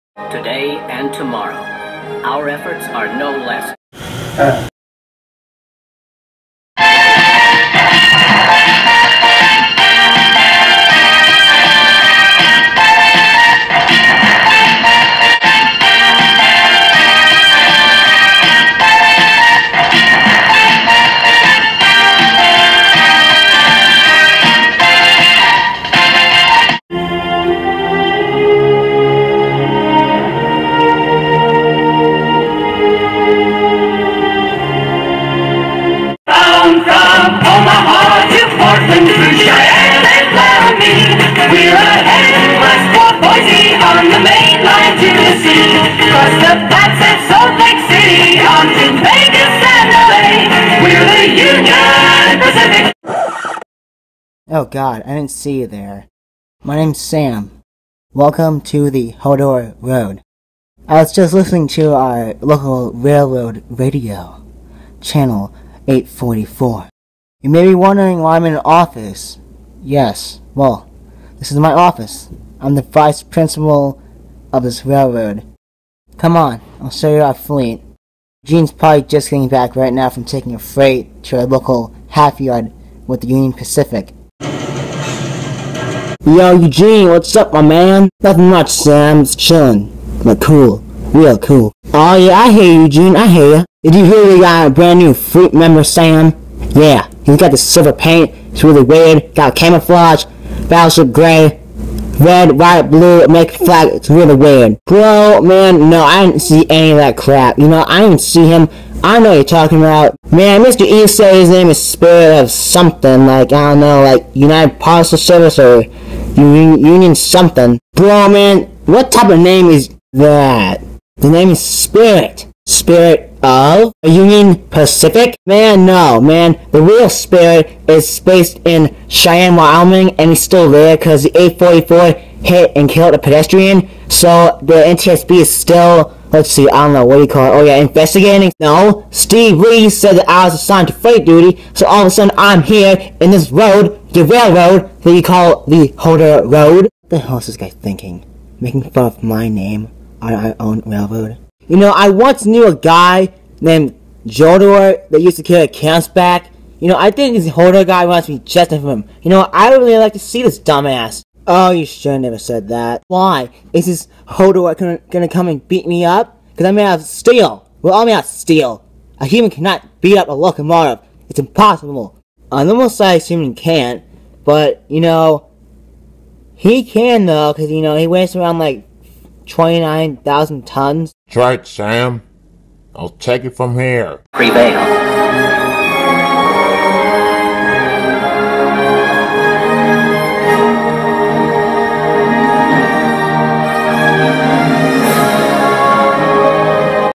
THE HODOR ROAD®™ episode 1 season 1 part 1
The first episode of THE HODOR ROAD™®, this episode introduces you to our vice principal of THE HODOR ROAD®™ and you also are able to meet three other of the various fleet members on the roster. New episodes are in production every day and are to be released by the end of Monday each week.

Dedicated to school staffs everywhere and their principles and vice principles